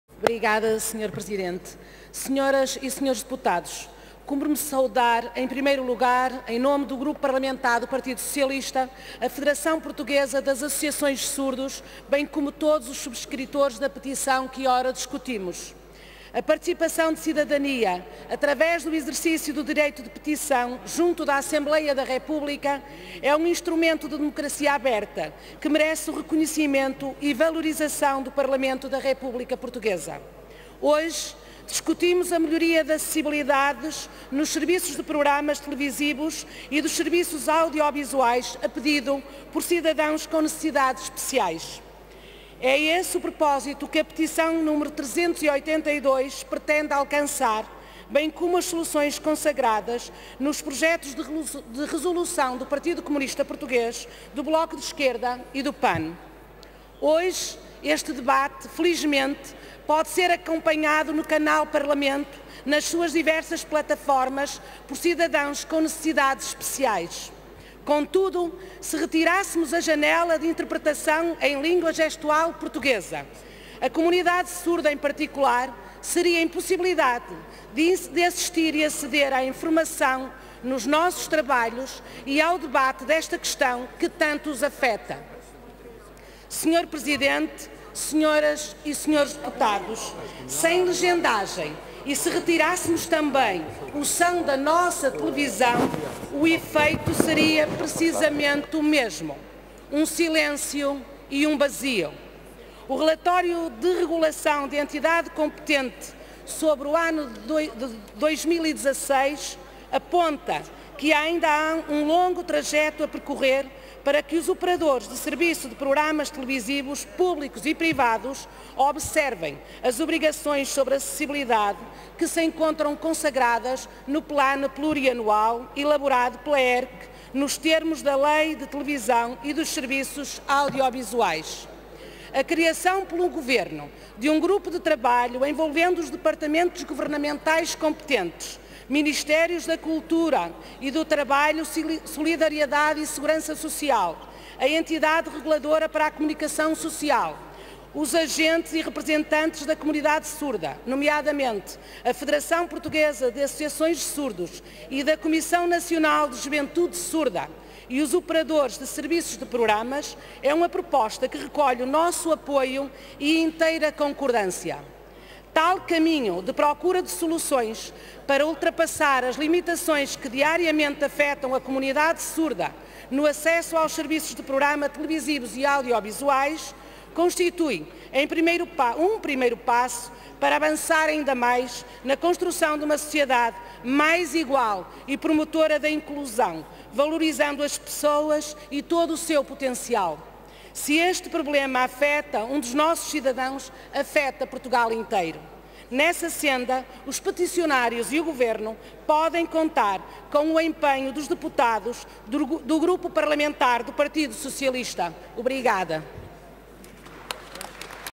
02-02-2018 - Debate Parlamentar | Acessibilidade televisiva para surdos Palmira Maciel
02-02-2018 | Intervenção da Deputada Palmira Maciel, no âmbito da Petição e Projetos de Resolução (PCP, BE e PAN) Criação de melhores condições de acessibilidade televisiva para a população surda portuguesa